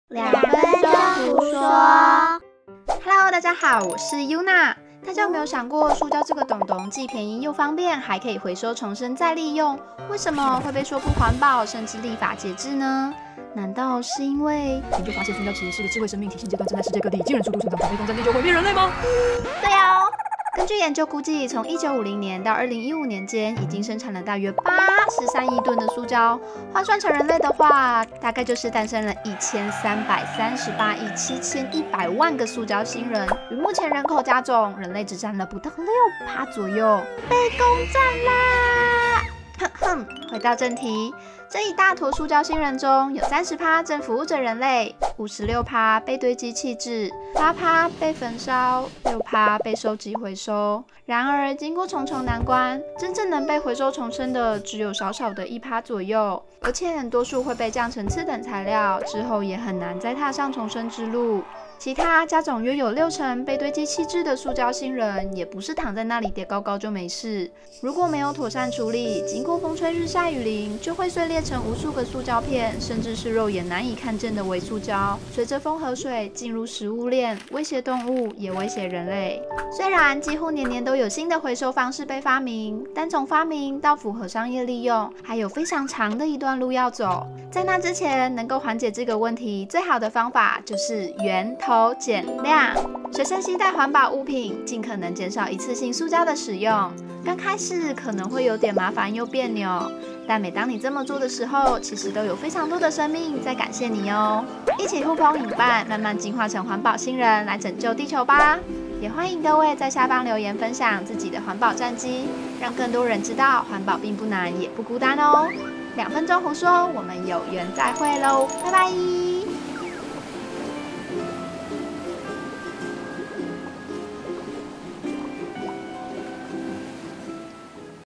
只有 1 %的塑膠有被好好回收 ?! 【2分鐘who說】
► 如果你喜歡這部影片的話，可以來給我一點點的贊助與鼓勵噢(*´▽`*)♥!

咦 ? 塑膠明明就可以回收再利用，為什麼要減少塑膠使用呢 ?
塑膠星人又是怎麼一回事阿 ?? 塑膠真的會危害地球嗎 !!??
今天就來一探究竟吧😆 !

【題外話】
台灣雖然是號稱垃圾回收率高達50%，全球數一數二的回收資優生
但這個「垃圾回收率」的算法是將「收集到的可回收垃圾總重÷收到的垃圾總重」，回收物們有沒有成功被轉換利用或是妥善處理，並無法從這個數據得知噢！

▶想了解更多海洋與塑膠資訊可以看這邊
塑殺事件簿
失控的海洋廢棄物──塑膠砌成的海龜墳場

▶影片主要資料來源
Production, use, and fate of all plastics ever made